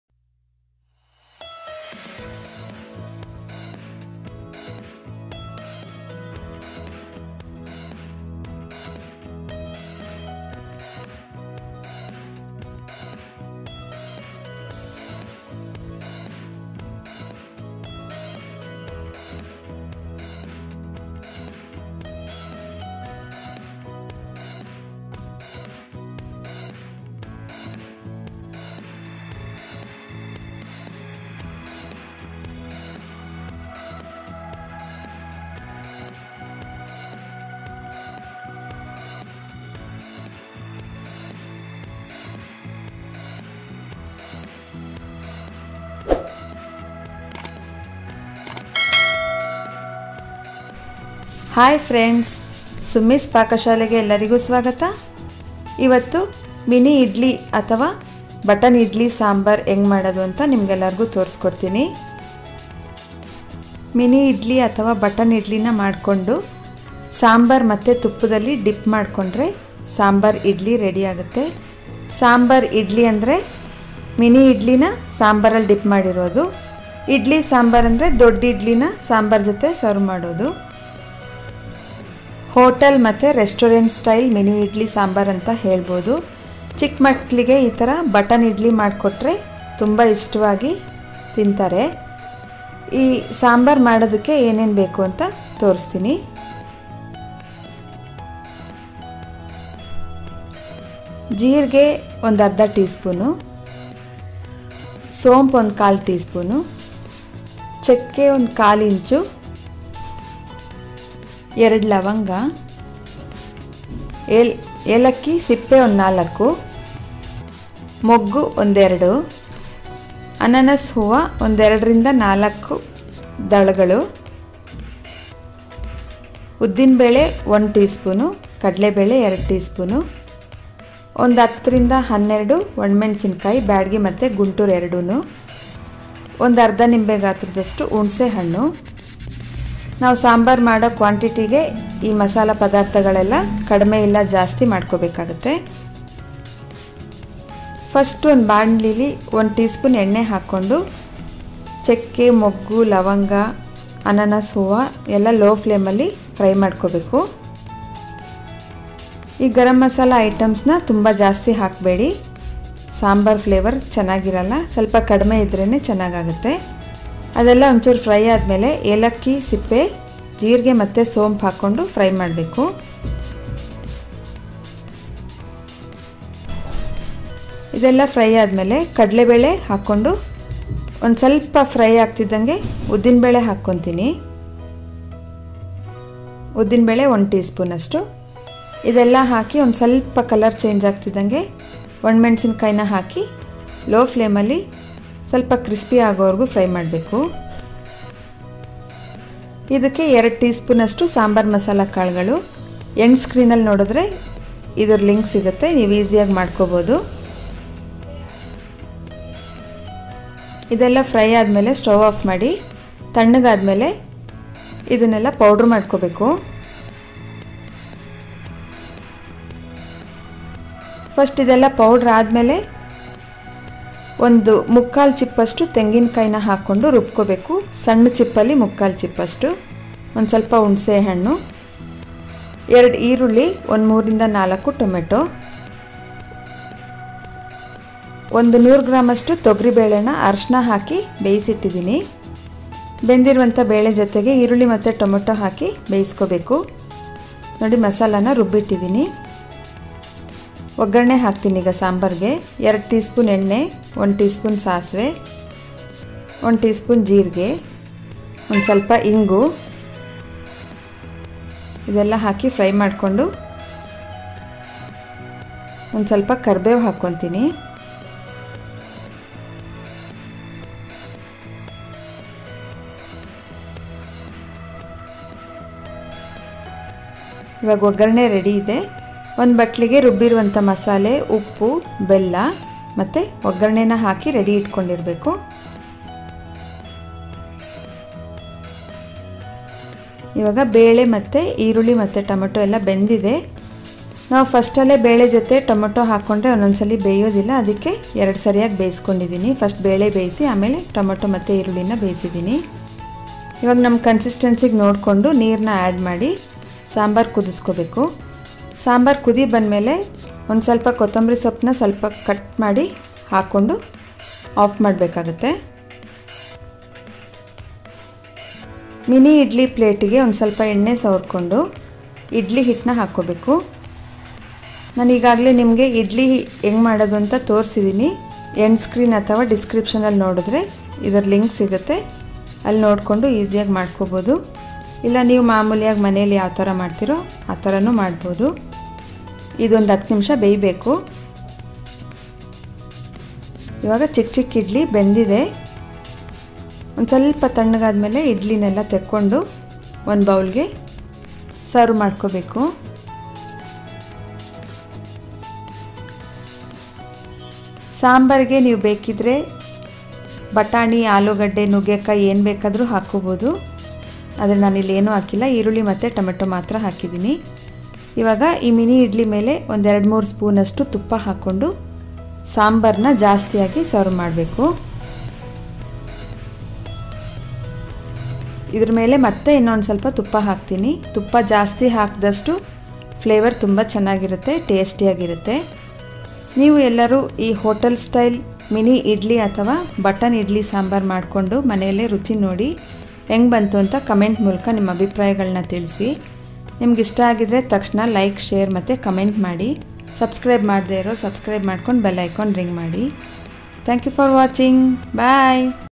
Button/Mini Idly Sambar | Restaurant Style| Simple and easy
SAMBAR IDLY / MINI  OR BUTTON IDLY SAMBAR

Onion - 2
Tomatoes - 3 - 4
Toor dhal  -1/3cup
Termuric -1/8tsp
Water

Cinnamon -1/4inch
Kapok bud/moggu -1or2
clove -2
Pineapple flower/ananas hoovu -2petal

Cardamom Peel - 4
Jeera - 1/2 tsp
Sounf- 1/4 tsp
Urad Dhal - 1 tsp
Channa Dhal - 2 tsp
Red chillies - 10-12
Sambar Masala seeds - 2 tsp
Coconut - 1/3 cup
Tamarind
Jaggery
Salt

Regular Idly batter for mini or button idly.